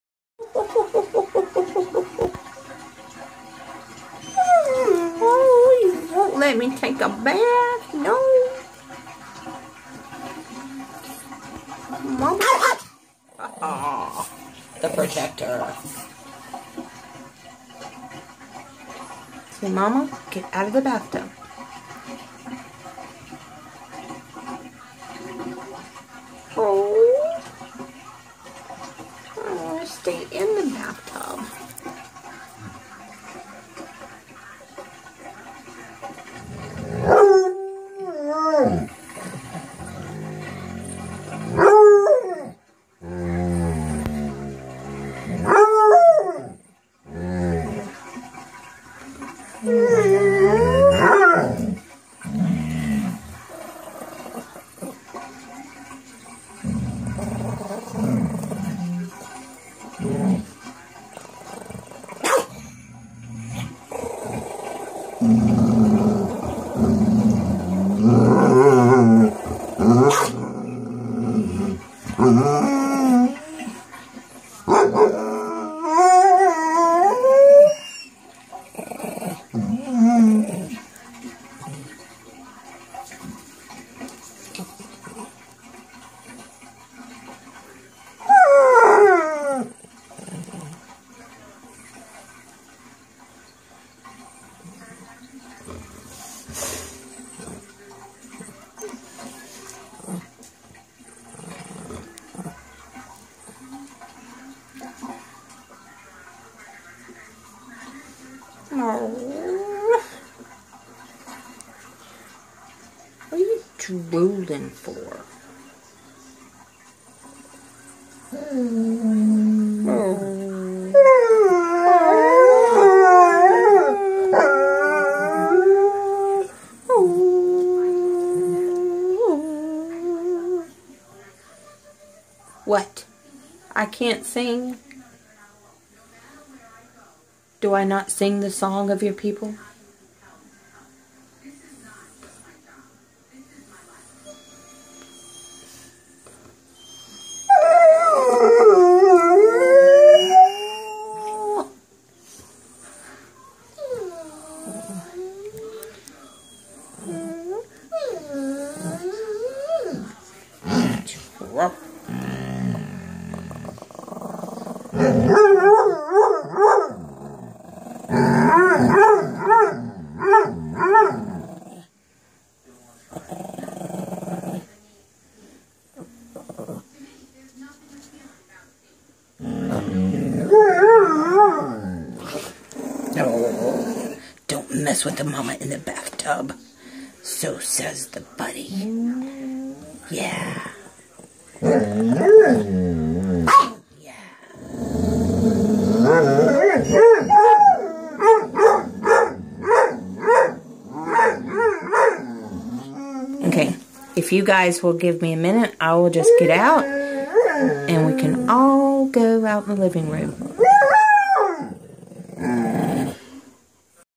Showdown at the OK BATHROOM
Two Shar Pei mixes vs one leetle chihuahua